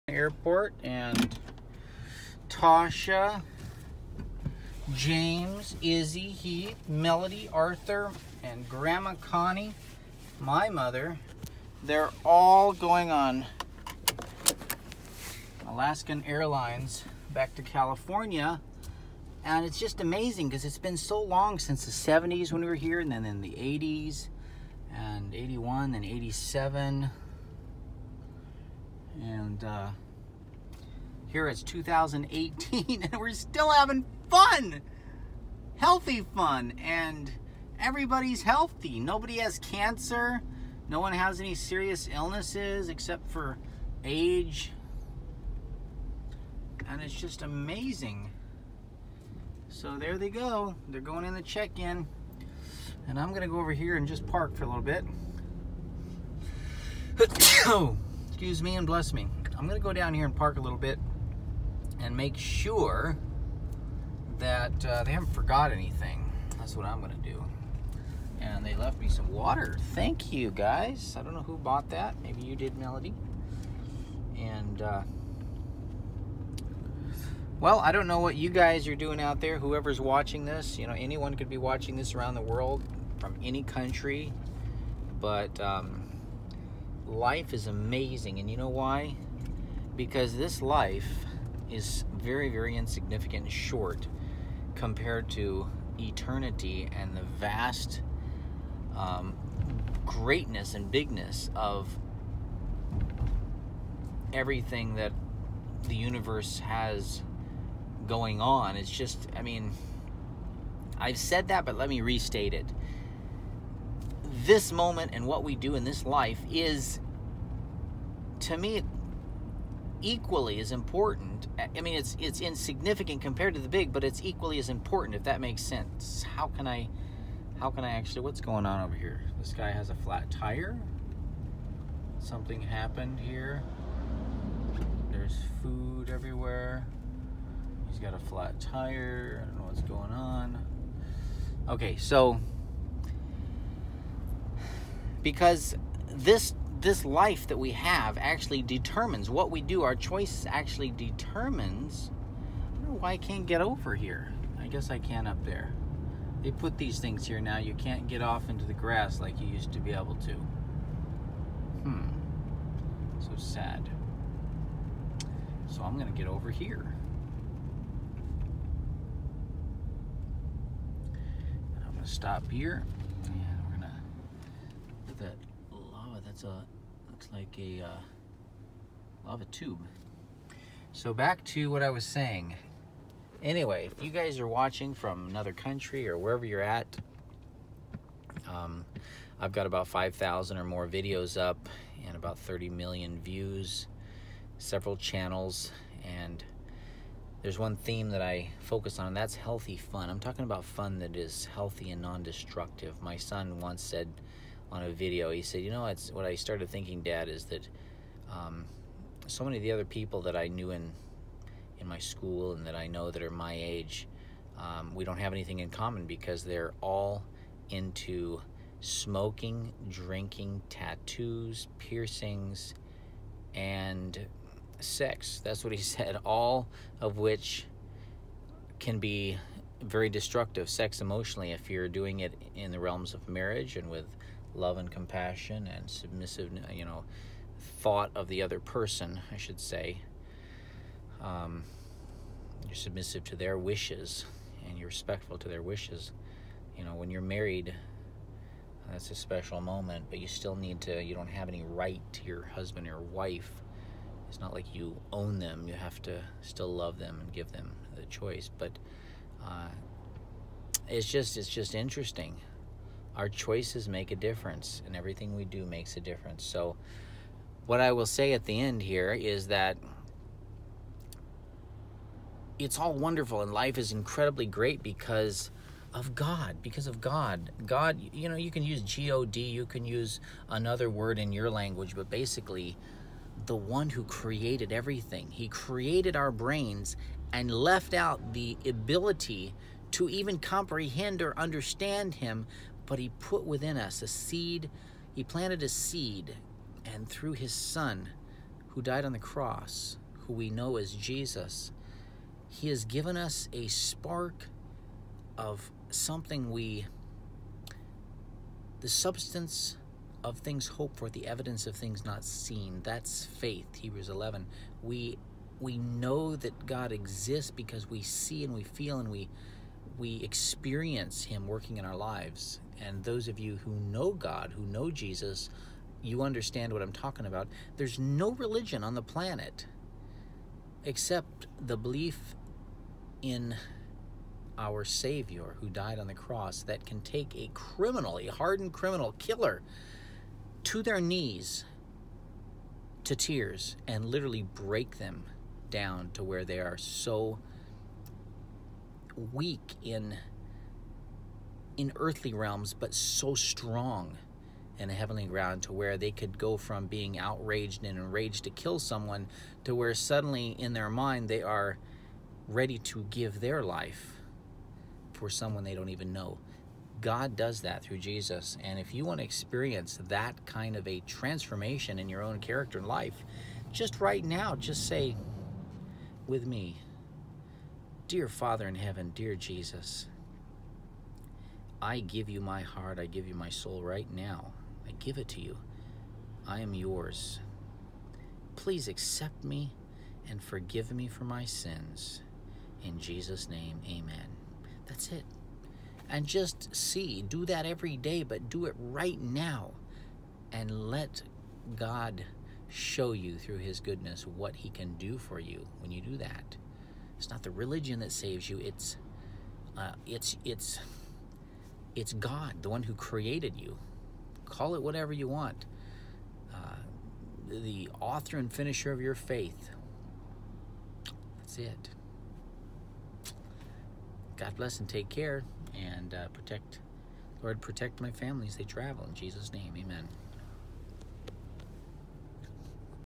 Johnson’s & Koerner Family Leave Kona
Steven Jenkins talking about life about God about what matters and healthy fun 2018